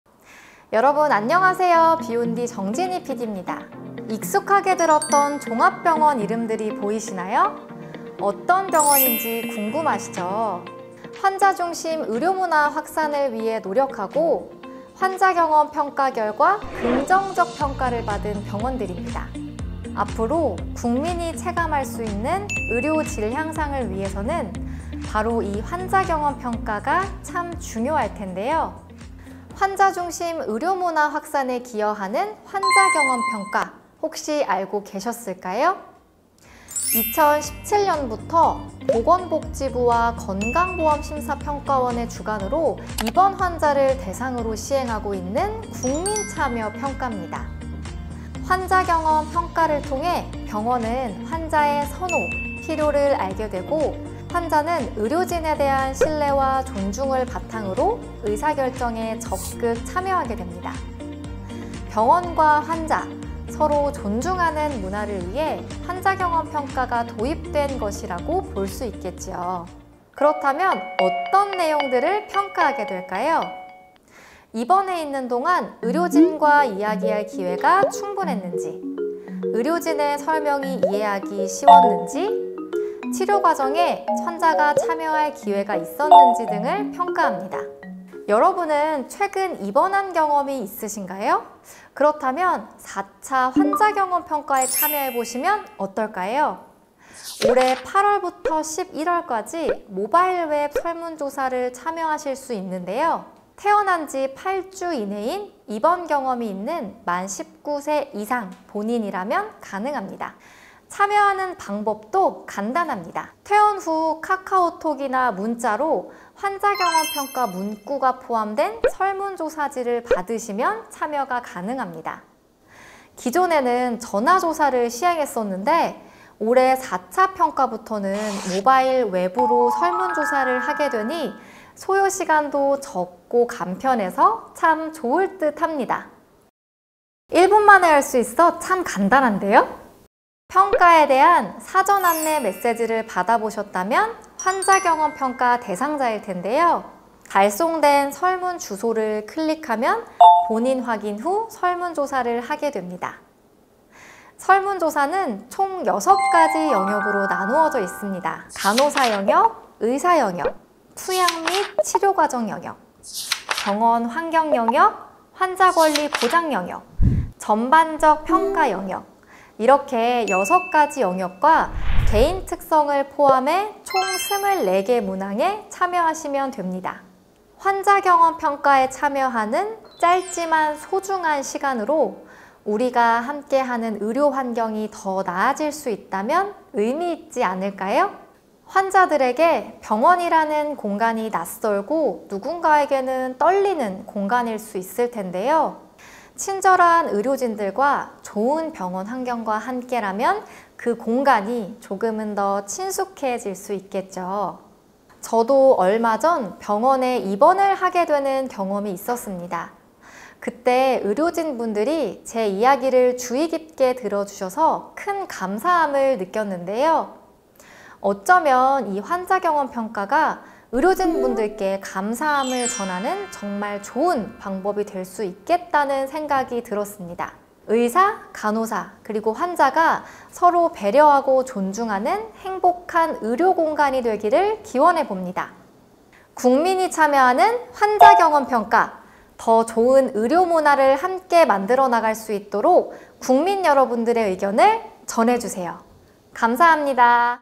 더 좋은 의료문화를 위해! 국민이 직접 참여하는 2023년(4차) 환자경험평가
국민이 직접 참여하는 '환자경험평가', 알고 계신가요?

✔ 환자경험평가
보건복지부와 건강보험심사평가원의 주관으로 국민이 체감할 수 있는 의료 질 향상을 위해
- 입원 기간 동안 의료진과 이야기할 기회가 충분했는지
- 의료진의 설명이 이해하기 쉬웠는지
- 치료 과정에 환자가 참여할 기회가 있었는지 등을 평가합니다.

✔ 참여대상
종합병원 이상 병원에서 1일 이상 입원했던 만 19세 이상 환자입니다.

✔ 참여방법
올해 8월부터 11월까지 카카오톡 또는 문자 등
모바일웹 조사로 참여가 가능합니다!

간편한 참여로 진료 과정에 환자의 가치와 의견이 반영되는
환자중심 의료문화를 만들어 보는것은 어떨까요?

더 좋은 의료문화를 위해 여러분의 의견을 전해주세요!

00:00 인트로
00:29 환자경험평가란?
01:10 평가내용
01:36 참여대상, 조사기간, 조사방법, 대상기관
02:19 모바일웹 조사 참여방법
02:33 평가영역
03:08 마무리